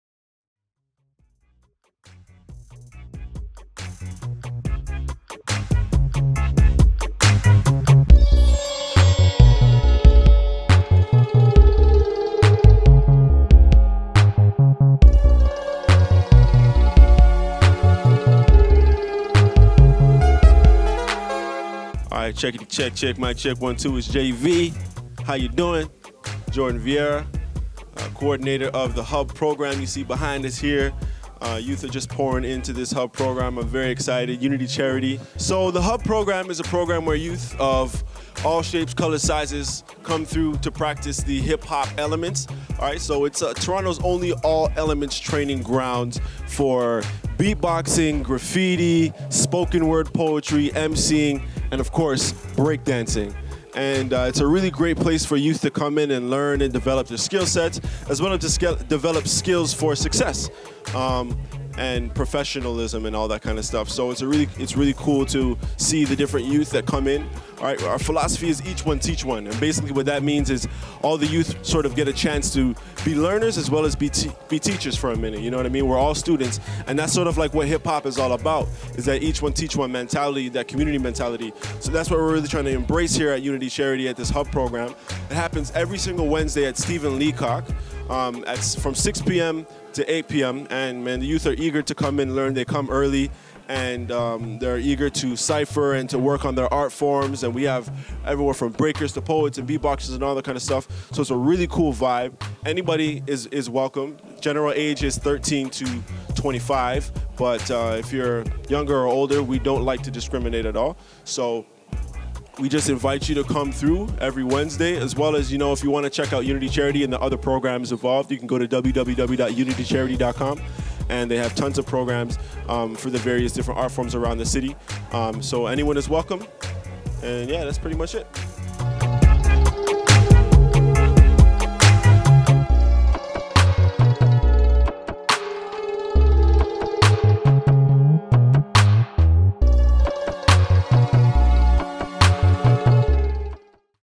Unity Hub @ Stephen Leacock C.I. [2014]
Toronto's first all elements hub featuring poets, b-boys/b-girls, graffiti artists, spoken word and more! Be sure to come through to learn, cypher and be a part of this weekly hip hop movement!

Every wednesday on the second floor staff lounge at Stephen Leacock Collegiate (2450 Birchmount Road, Toronto) in the Steeles L'amoreaux Community. Artists of all type will come from all over the city to work on their hip-hop based art forms, and develop skills for success! This is a HUGE opportunity if you are a an aspiring artist and are passionate about taking your craft to the next level!

Directed by Crystal Media
Produced by SLYE Network
Shot by Shahaab S. x Hassaan K.
Edited by Shahaab S.